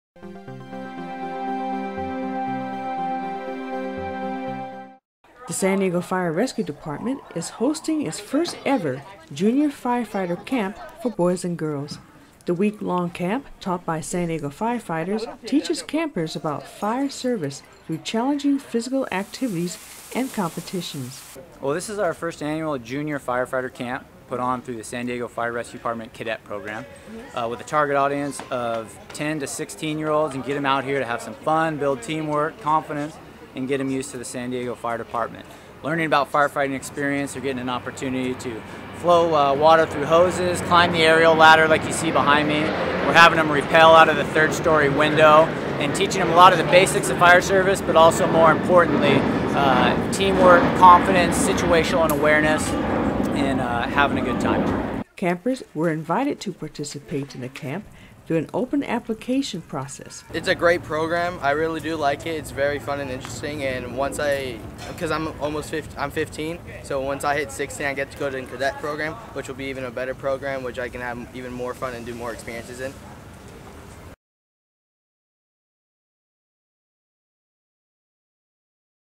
San Diego Junior Firefighter Camp
San Diego Fire-Rescue Department hosted its first-ever Junior Firefighter Camp. Boys and girls in this week-long camp learn about the fire service through challenging physical activities and competitions.